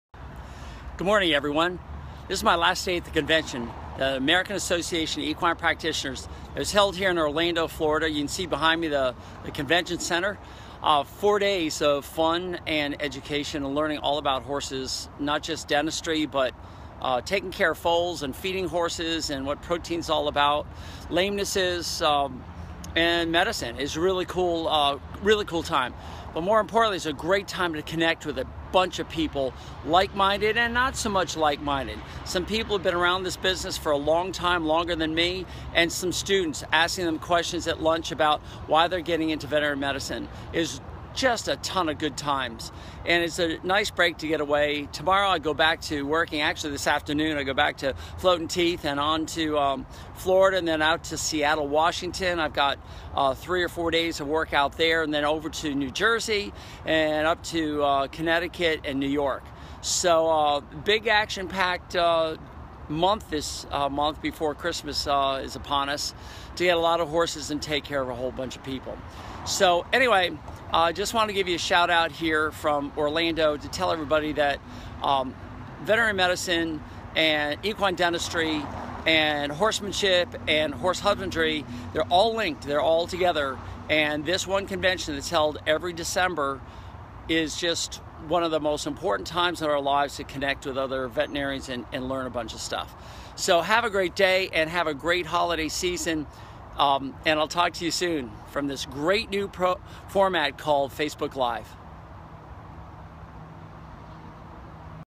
Doc T at the American Association of Equine Practitioners annual meeting
Doc T using Facebook Live - from Orlando, FL at the AAEP convention (American Association of Equine Practitioners)
FBL 161207 DocT AAEP first FBL